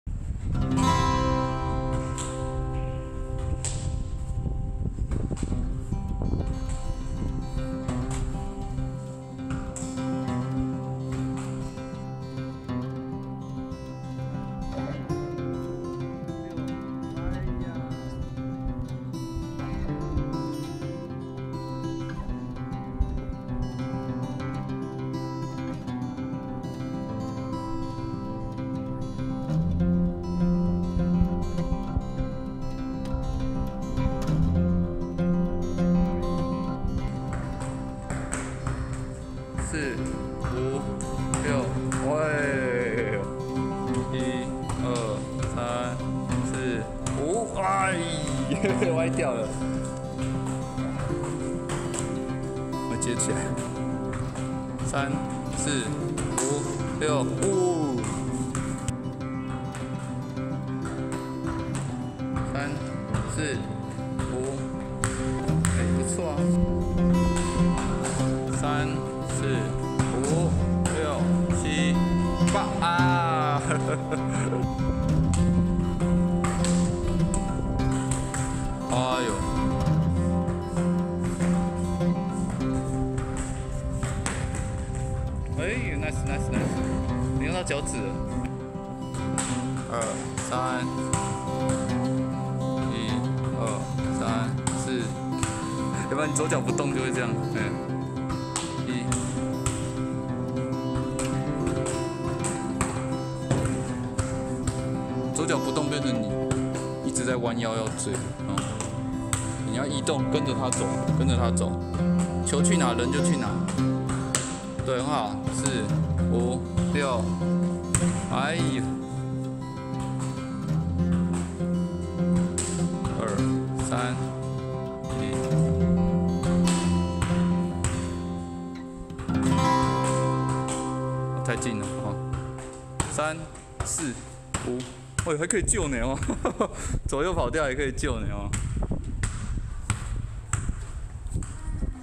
一對一教學紀錄---這個學生今天超想練挑球~我當然是求之不得呀!
今天帶學生練習時~他突然超想練挑球
所以最後放給他半小時練習
看起來是蠻有天份的...應該很快就能衝起來了
很久沒看到這麼有耐心的小朋友了